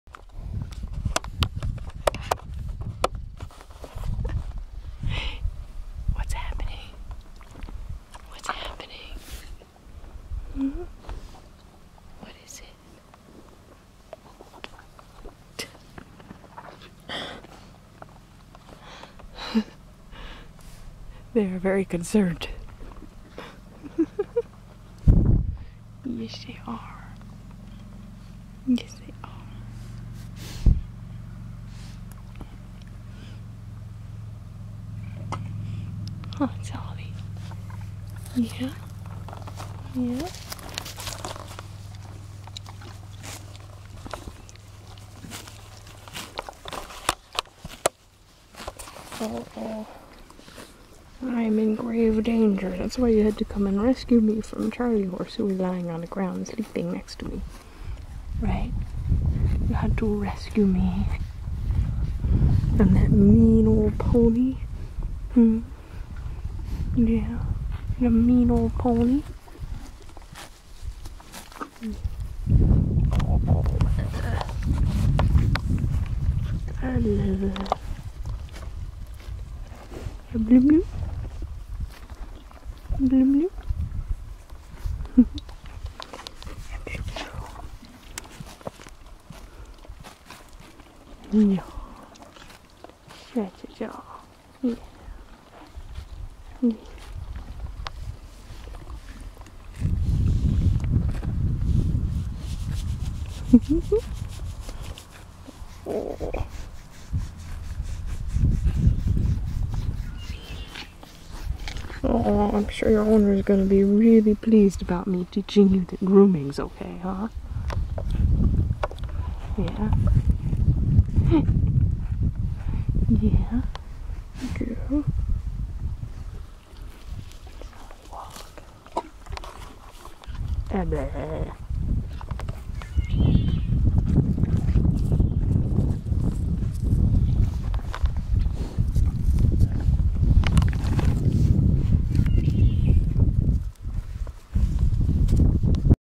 Maggie and Solomon to the rescue!
I sat down next to Charley Horse, the palomino pony.  he was taking a nap. This was cause for Much Concern for Solomon and Maggie, who were convinced that I might get rolled over on, or, even worse, might give skritches to SOMEONE ELSE.  This is right after they chased him off, heh!